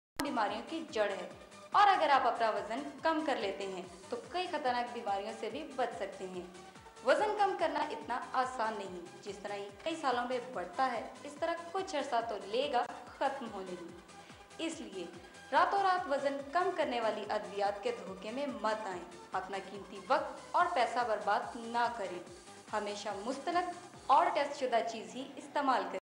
The K.S.D Girl
This anonymous Pakistani model is selling one of those, late-at-night, weight losing miracle drugs.